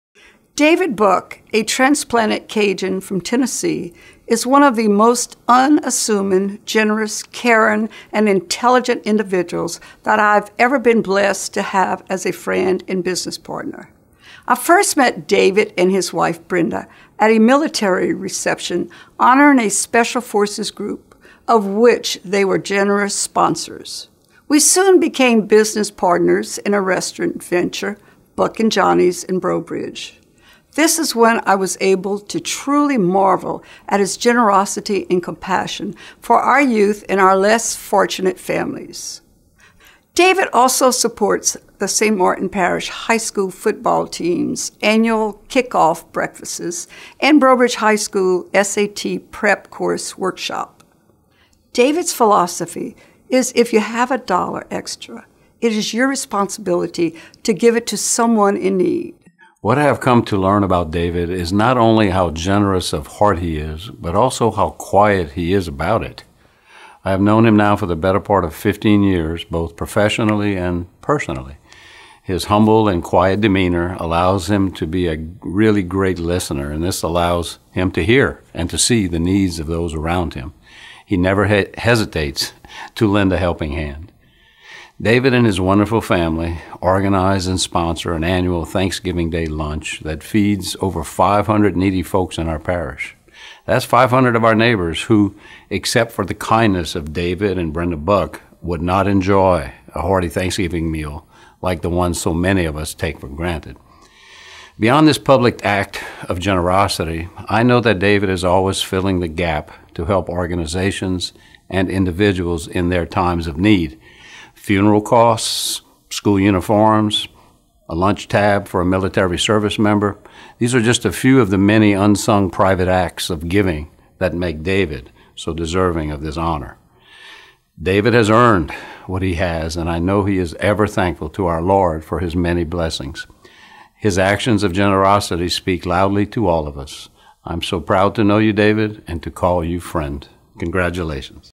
David Buck
David Buck received the 2015 Leaders in Philanthropy Award from Community Foundation of Acadiana for St. Martin Parish.